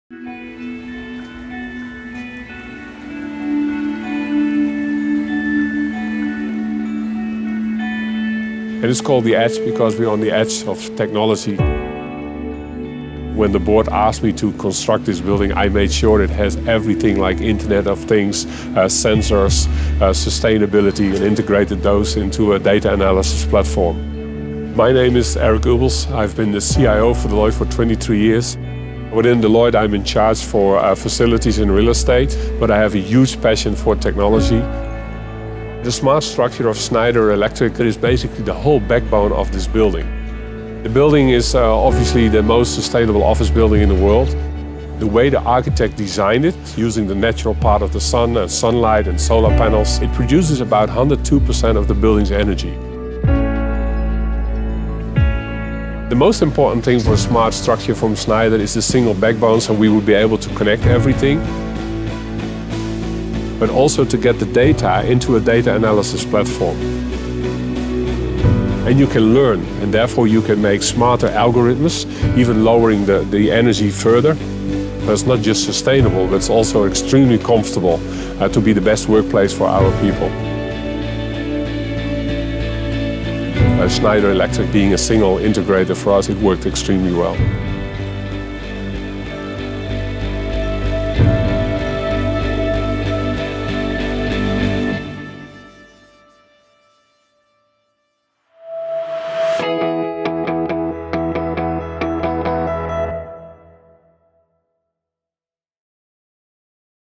Discover EcoStruxure - Schneider Electric & Deloitte - Greater Than the Sum of its parts
Building the world’s most sustainable office building involves leveraging the best and the latest technologies. The Edge in Amsterdam, head office of Deloitte Netherlands knows this first hand. Using a range of Schneider Electric offers, including its SmartStruxure™ solution, they have succeeded in setting a new global benchmark for energy efficient commercial office environments while prioritizing the satisfaction and well-being of its employees. Special recognition to the valued partners who also helped make The Edge a success: HC Groep, OVG Real Estate, PLP Architecture, HC Barcol Air, Deerns, and Homij Bosman Combinatie (HBC).